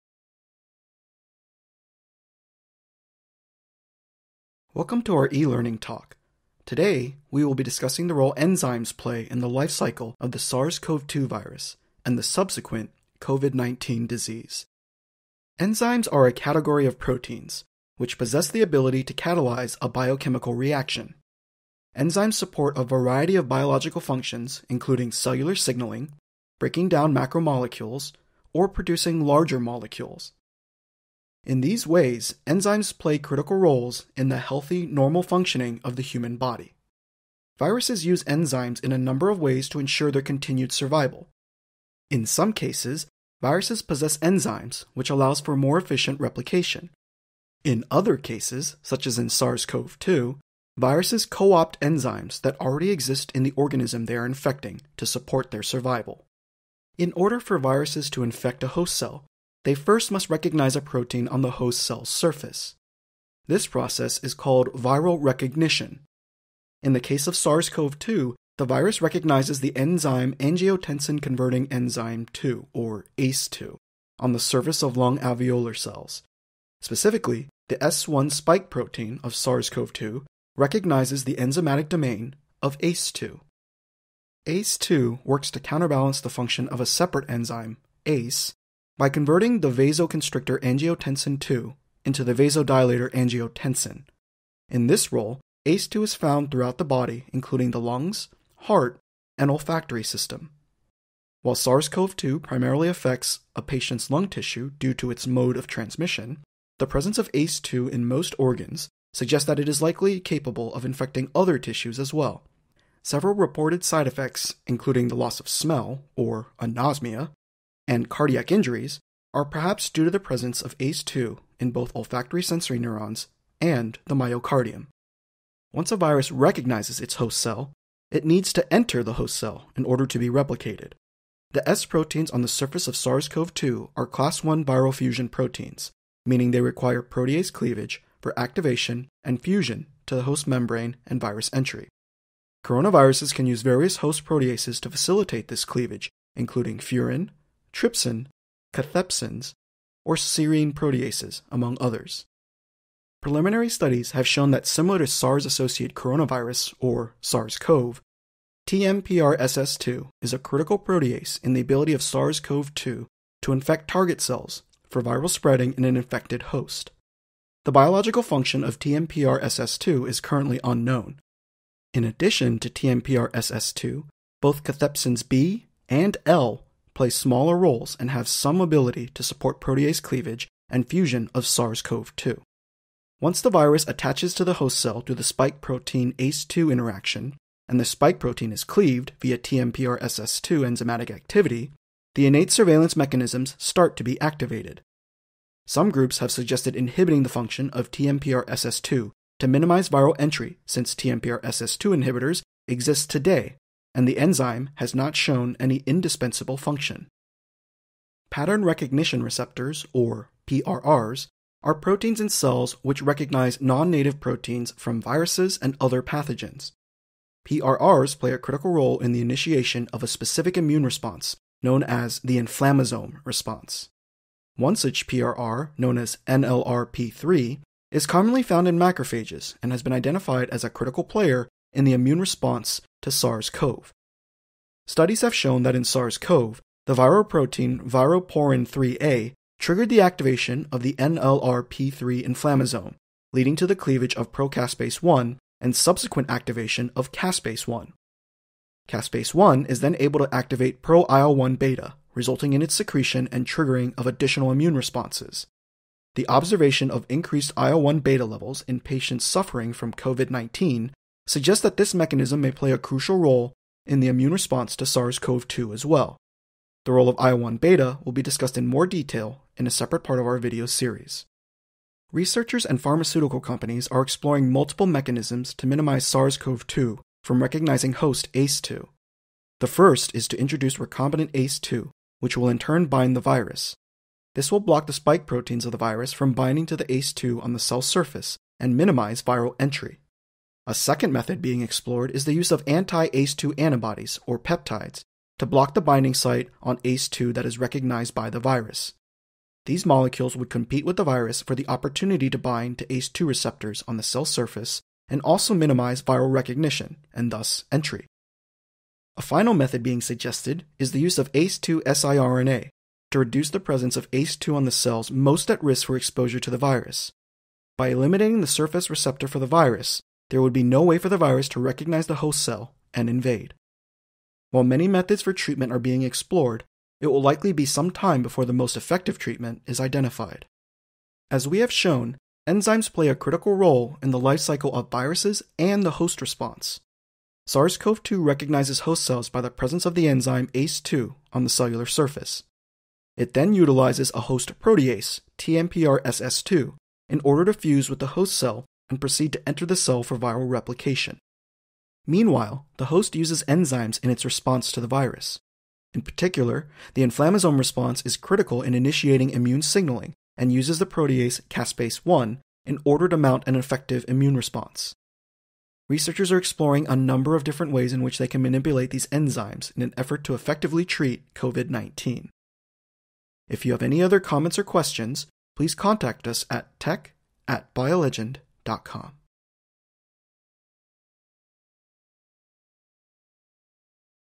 Enzymes in COVID-19 Inflammation
Enzymes catalyze a number of important biological reactions. In the context of SARS-CoV-2, they are vital for viral entry and replication. Learn more about the key enzymes utilized by this virus and potential therapeutics to block them.